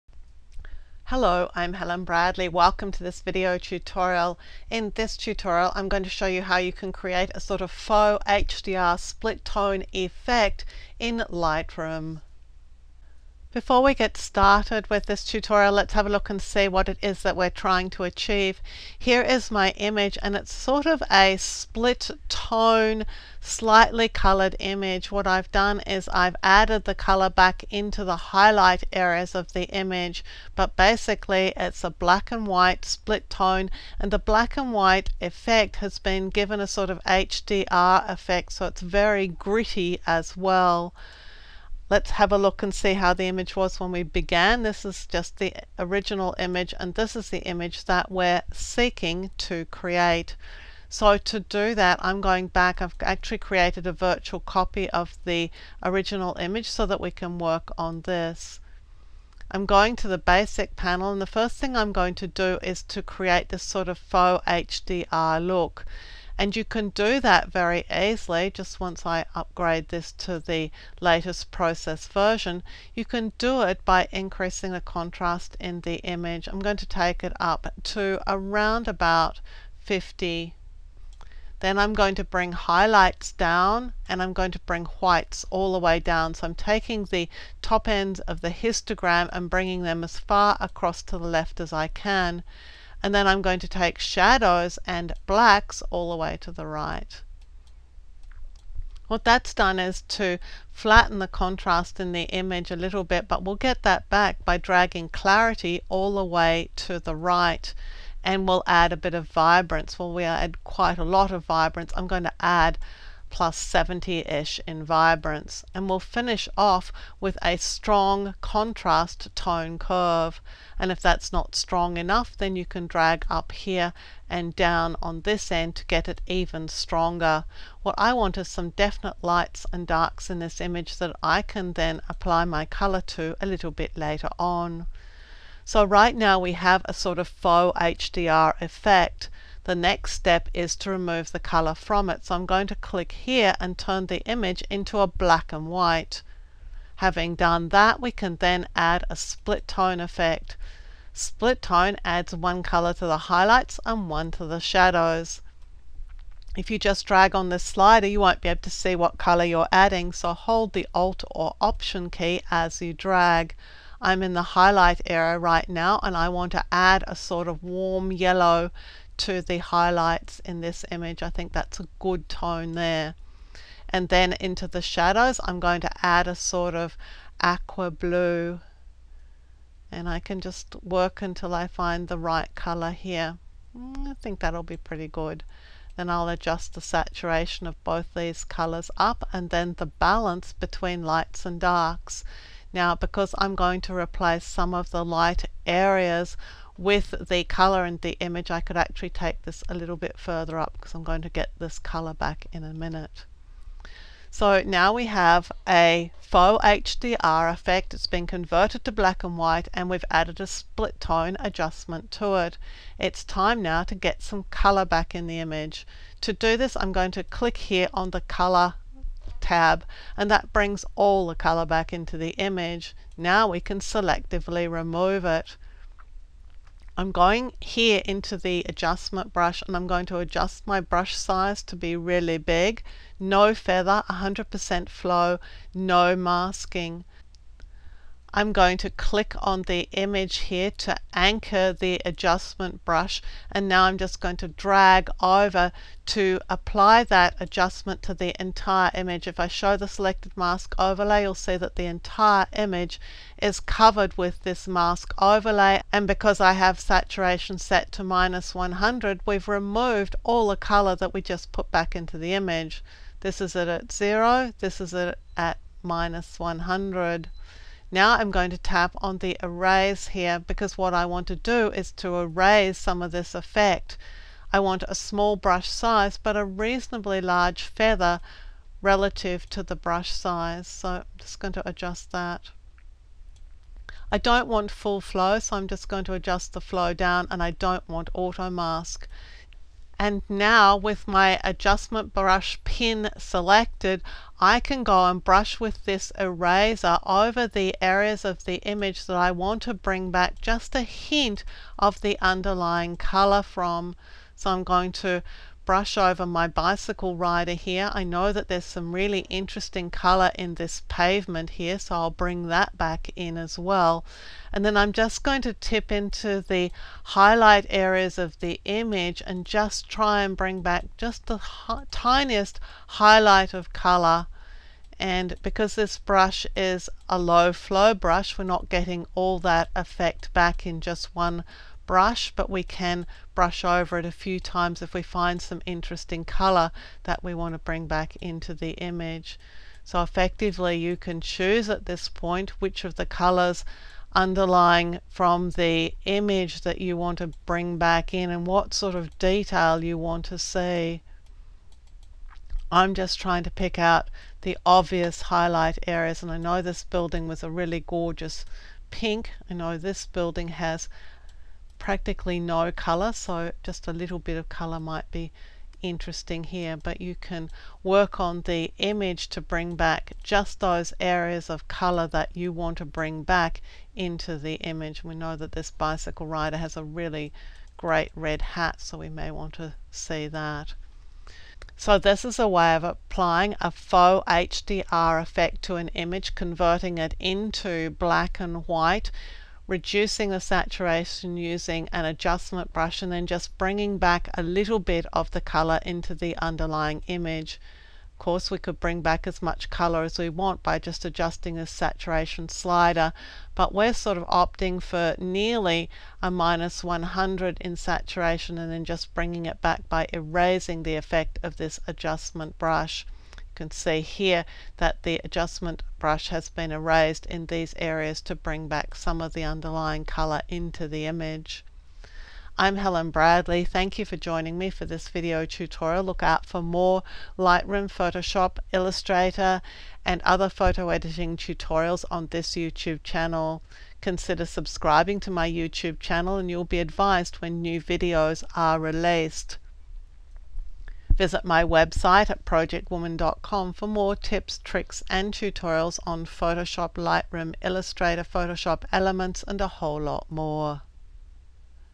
Create a Faux HDR Split Tone Effect in Adobe Photoshop Lightroom
See how to create a faux HDR effect in Lightoom then convert it to monochrome, add a split tone to it and finally bring back just a little color into the image.